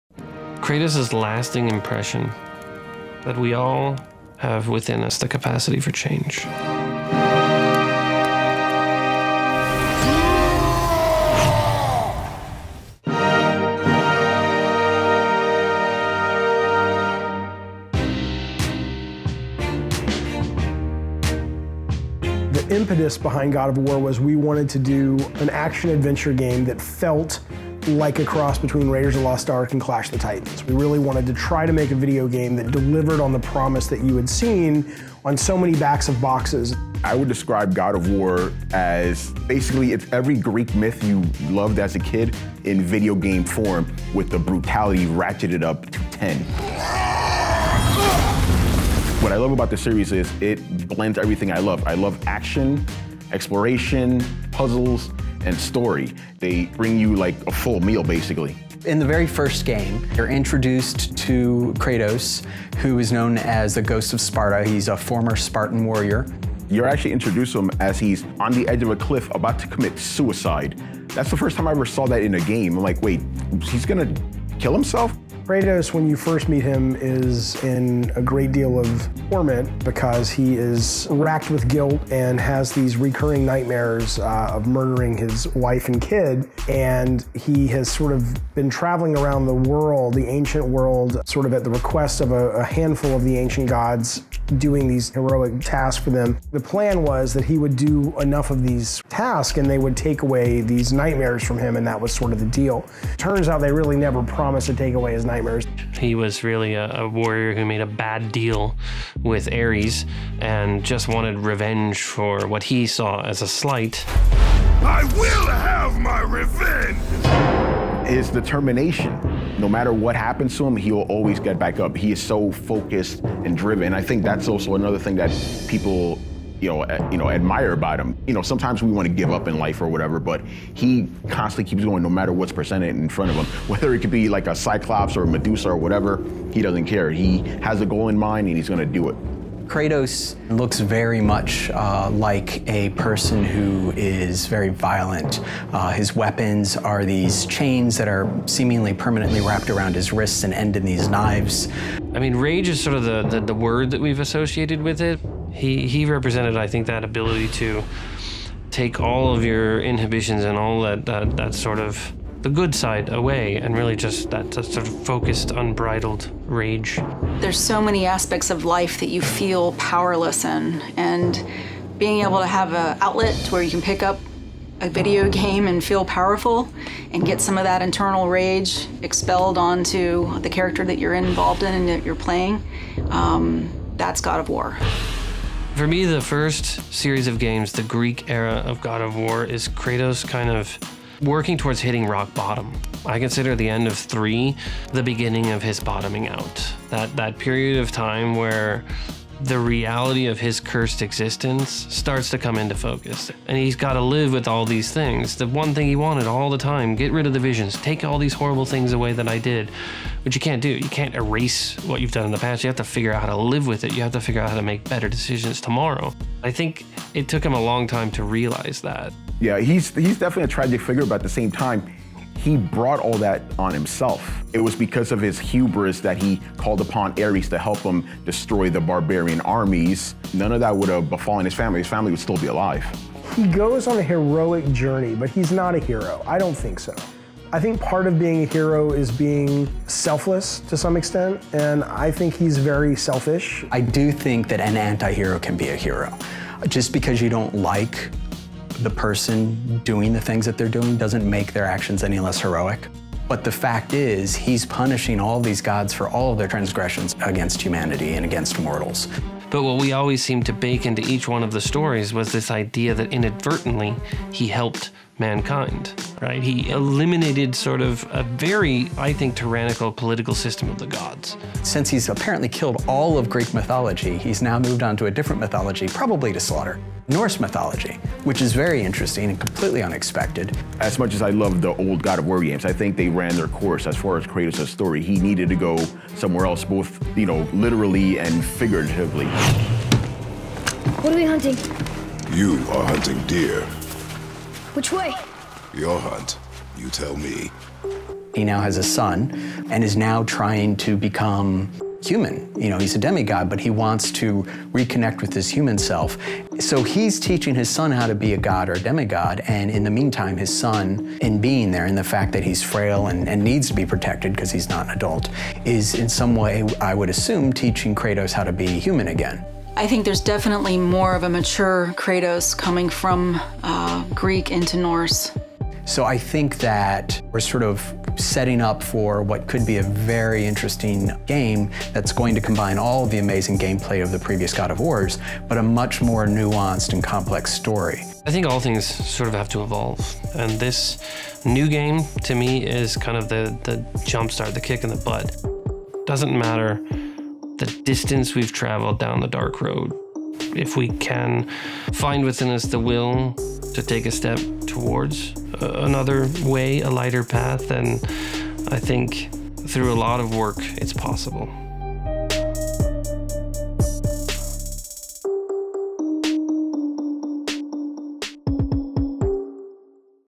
Heroes: God of War's Kratos
In God of War, we’ve seen Kratos get his revenge on the Greek gods, but is he a hero? Creators Cory Barlog and David Jaffe, along with gaming insiders, discuss the evolution of Kratos and where we will see him next.

Glixel is Rolling Stone's video game channel. We cover video games and the people who make and love them.